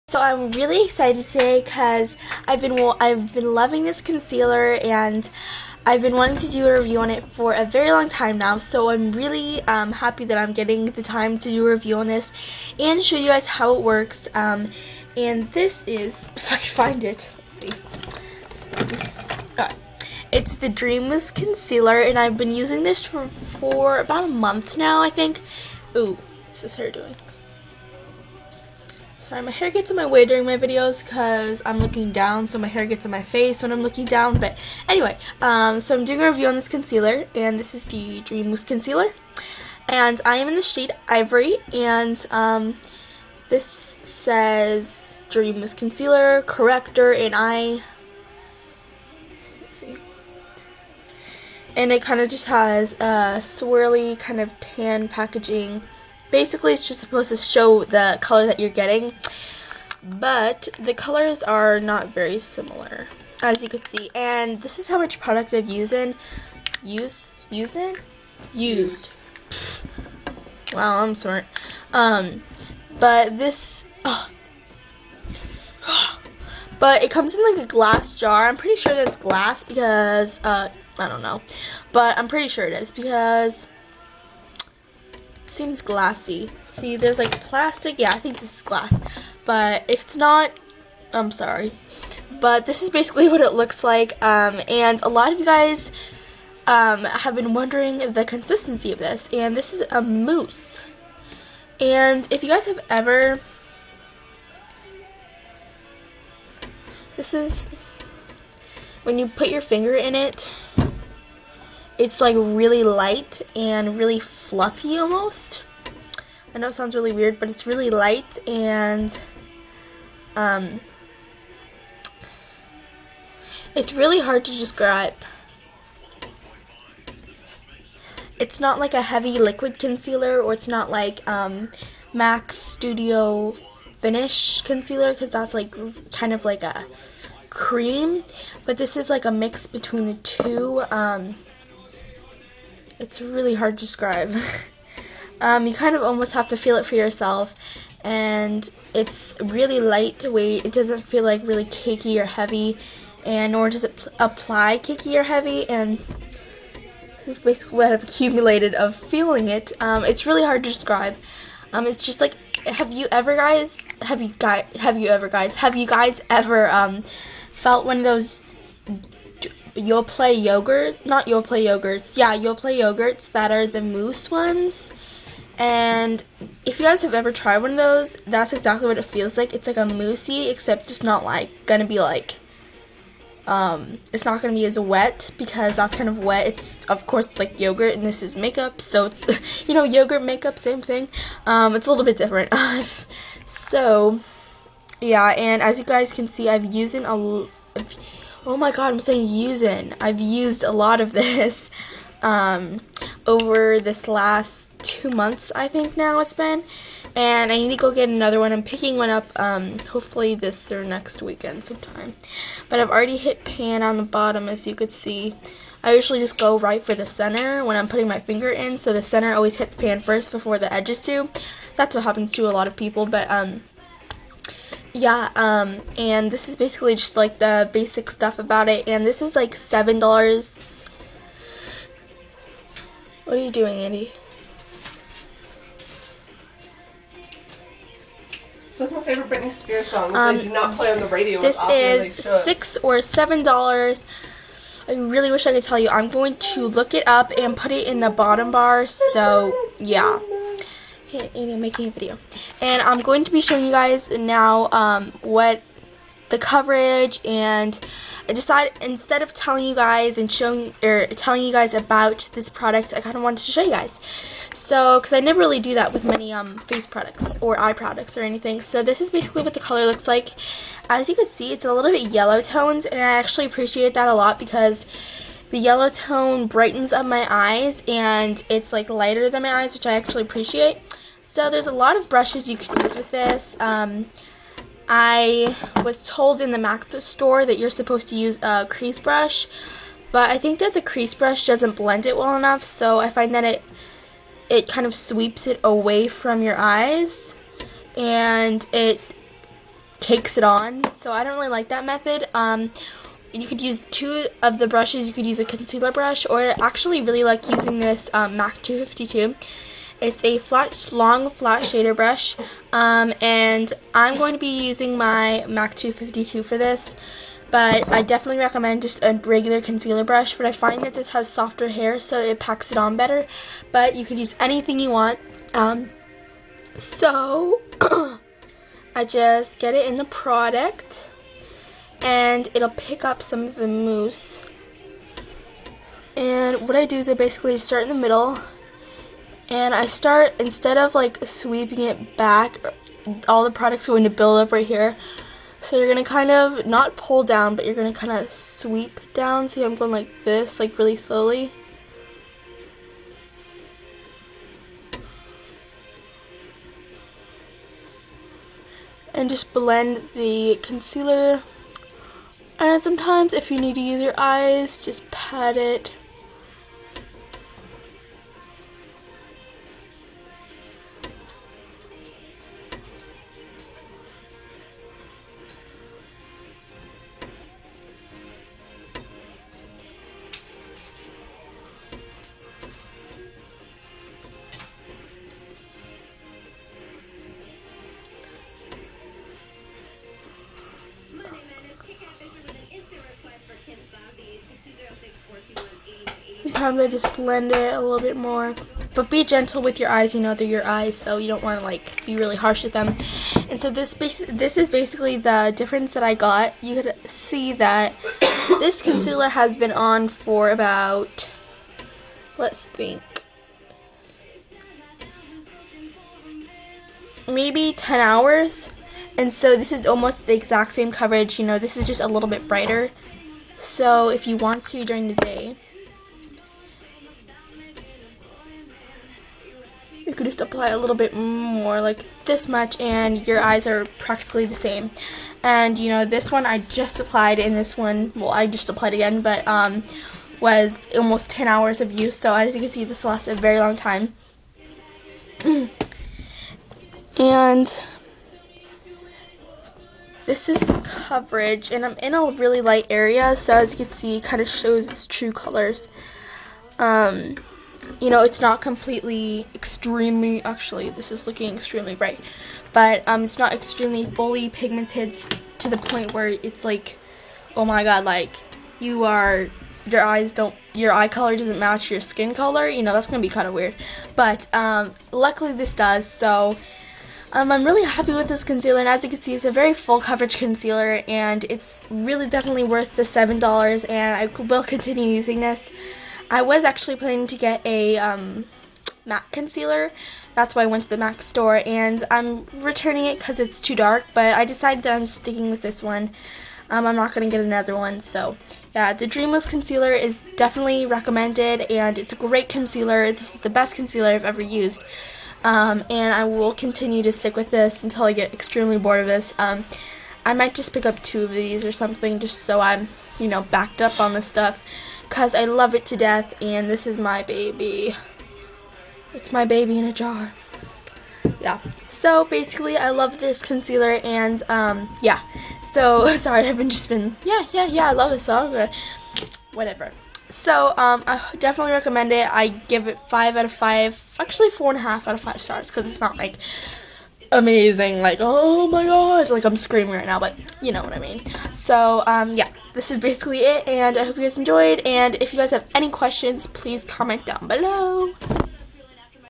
Dream Mousse Concealer Review+ Application
This is my favorite concealar and I definitely am repurchasing:) Any Questions? Comment Below.. PLEASE READ. I THINK THEY ARE BEING DISCONTINUED:((((( AS YOU CAN SEE SOMEONE SAID DOWN BELOW THAT THEY ARE AND I WAS AT THE STORE YESTERDAY AND THE SHELFS WERE PRACTICLY EMPTY. DARNIT...I NEED TO PICK UP ANOTHER ONE!!:( I DONT THINK A CONTST WILL BE HAPEENING IF THEY ARE DISCONTINED